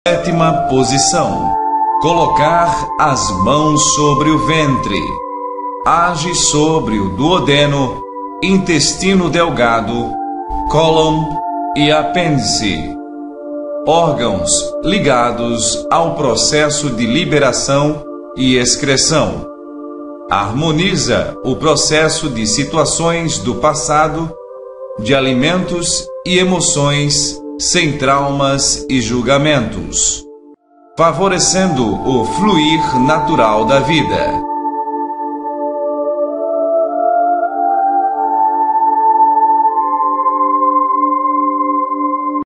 Reiki cura - posição 07
Serve para agir no intestino delgado, duodeno

Estamos desenvolvendo um conteúdo novo e de alta qualidade, contribua com uma doação no :